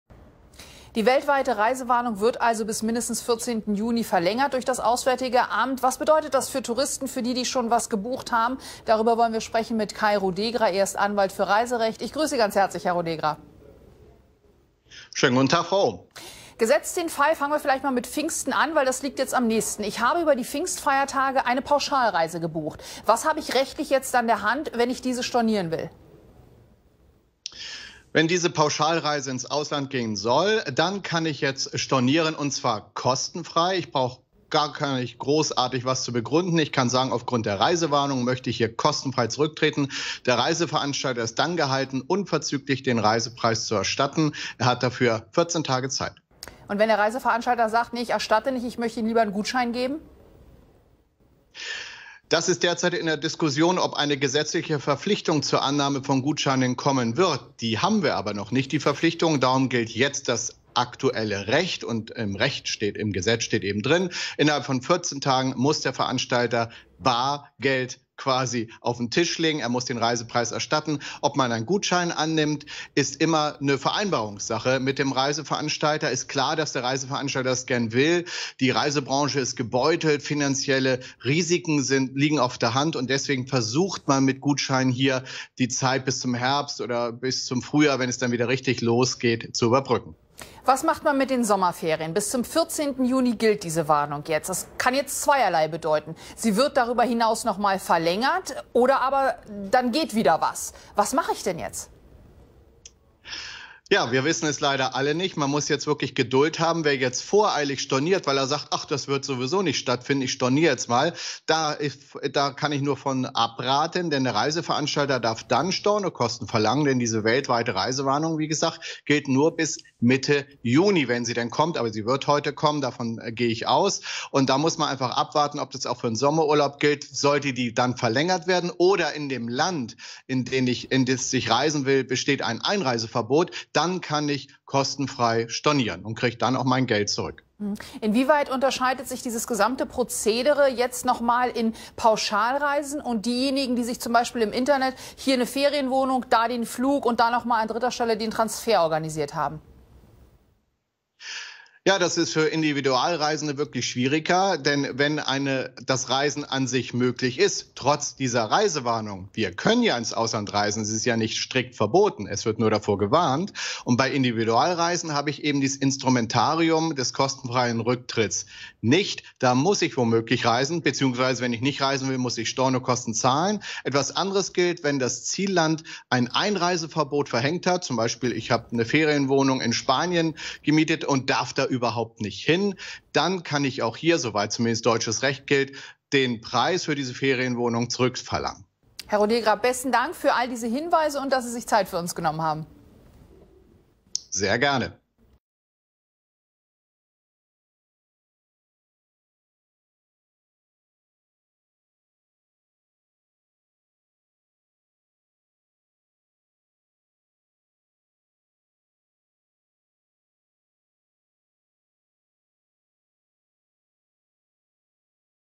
WELT INTERVIEW: Reisewarnung bis 14. Juni - Was heißt das für bereits gebuchte Reisen?
Die Verlängerung der weltweiten Reisewarnung macht Urlaub im Ausland in den kommenden Wochen praktisch unmöglich. «Urlauber haben jetzt bis 14. Juni weitgehend Rechtssicherheit», sagt der Reiserechtler Paul Degott aus Hannover. Doch was bedeutet das im Einzelnen für Urlaubspläne, die nicht mehr in Erfüllung gehen können?

Sollte ein Reiseveranstalter für einen Urlaub in den kommenden sechs Wochen eine ausstehende Restzahlung verlangen, können Kunden diese nun verweigern. «Viele Veranstalter sind ganz groß mit Mahnungen», sagt Degott. Doch die Reisewarnung gibt Sicherheit: Zahlungen für Urlaube mit Beginn bis 14. Juni müssen nicht mehr geleistet werden.

Viele Veranstalter bemühen sich derzeit mit Rabatten darum, Kunden zum Umbuchen anstelle von Stornos zu bewegen, damit das angezahlte Geld im Unternehmen bleibt. Tui zum Beispiel hat Kunden zuletzt bis zu 150 Euro geboten, wenn diese sich für eine Gutschrift entscheiden. Rein rechtlich haben sie weiterhin Anspruch auf eine Rückzahlung des Reisepreises.